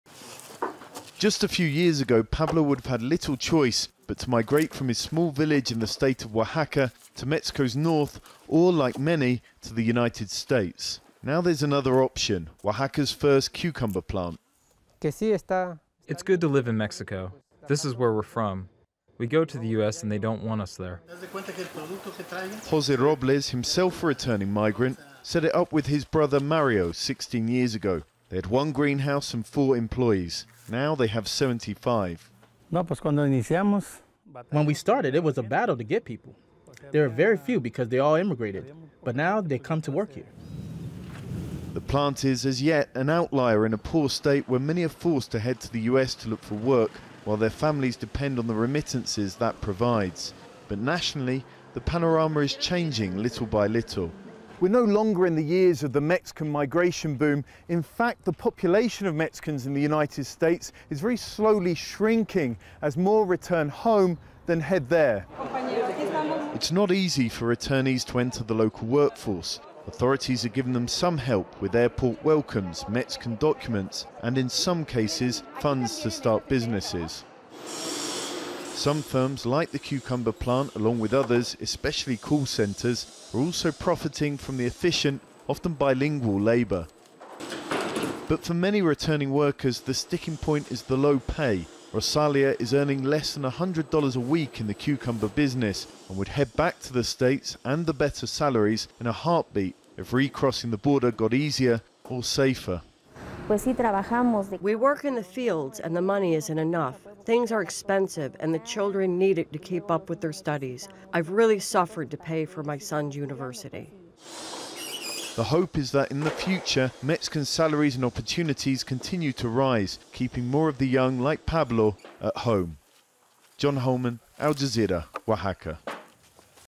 🇲🇽 🇺🇸 Number of Mexican migrants in US shrinking | Al Jazeera English
The number of Mexican migrants in the US is slowly shrinking.

A combination of long-term tough migration policies and increased border security has meant more people are returning home than are making the journey to the US.

Some analysts also believe that a slowly improving economy and more opportunities in Mexico are contributing to this.

Al Jazeera's John Holman has more from the rural, migration-dependent state of Oaxaca.